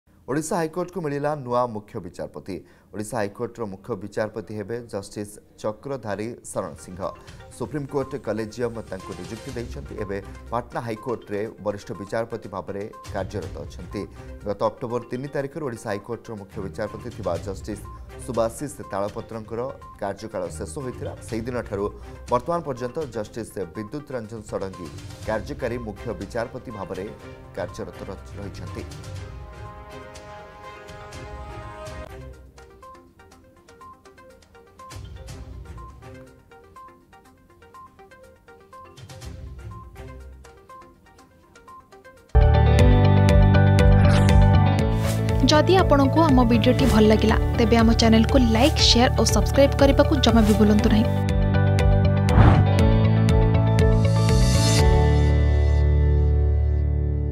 ଓଡ଼ିଶା ହାଇକୋର୍ଟର ମୁଖ୍ୟ ବିଚାରପତି ହେବେ ଜଷ୍ଟିସ ଚକ୍ରଧାରୀ ଶରଣ ସିଂହ || Chief Justice || Orissa High Court
ଓଡ଼ିଶା ହାଇକୋର୍ଟର ମୁଖ୍ୟ ବିଚାରପତି ହେବେ ଜଷ୍ଟିସ ଚକ୍ରଧାରୀ ଶରଣ ସିଂହ ।